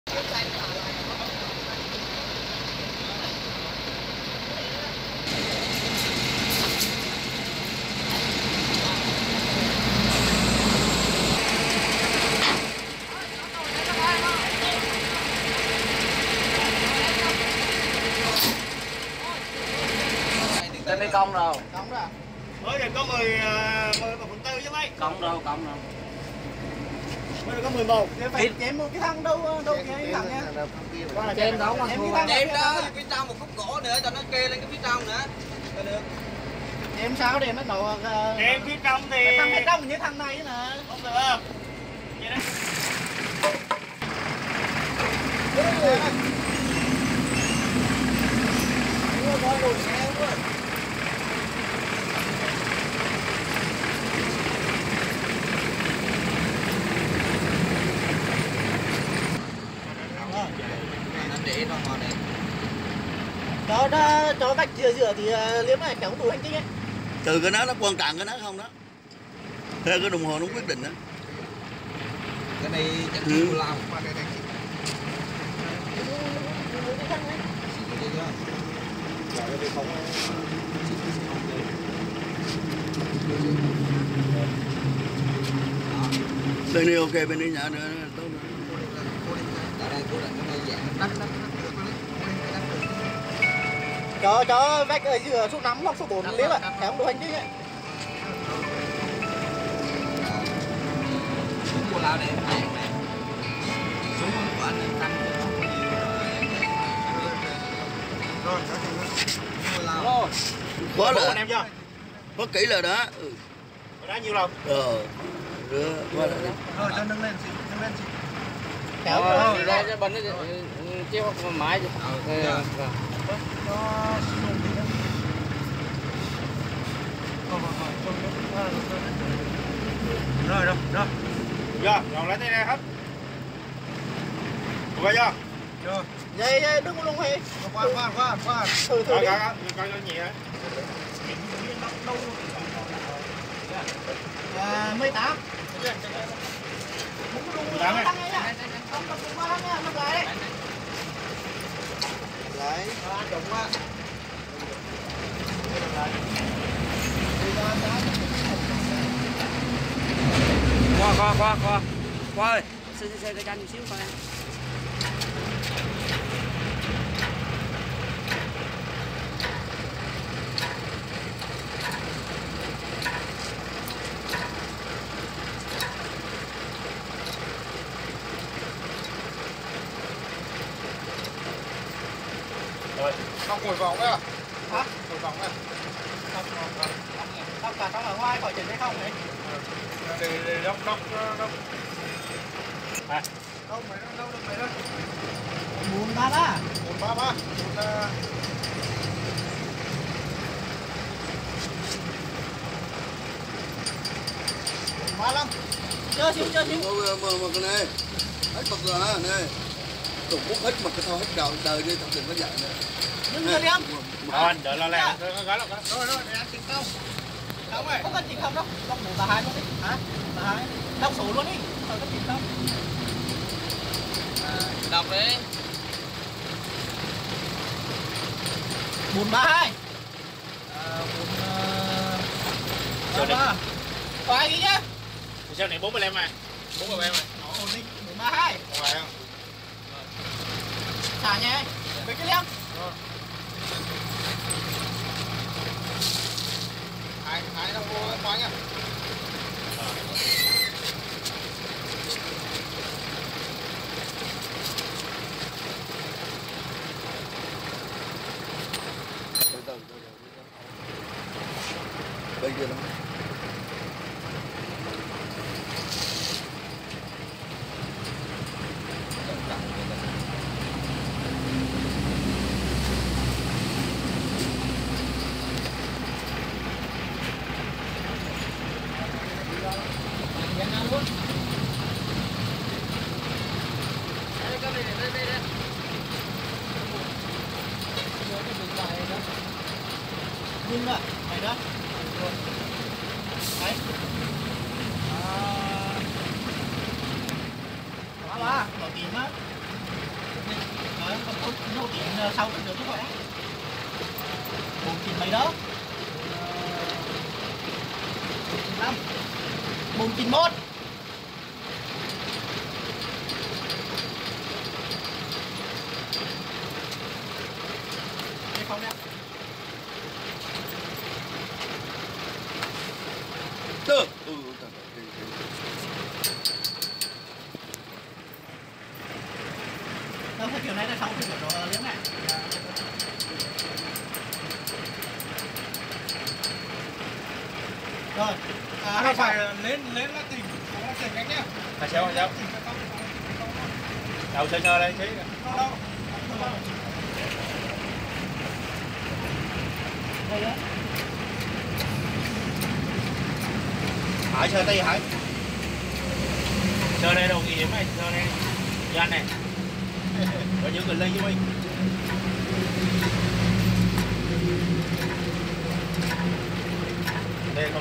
Thí nghiệm đo mô đun đàn hồi, bằng tấm ép cứng
Hình ảnh thực tế thí nghiệm đo mô đun đàn hồi nền đất K98, bằng tấm ép cứng